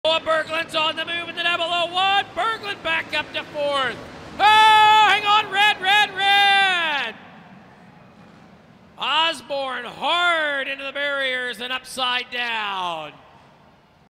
Kolton Osborn Crashes In Semi Pro Kart A-Feature in "Battle at the Barn"
All credit for footage goes to FloRacing. You can watch the full broadcast of the Battle at the Barn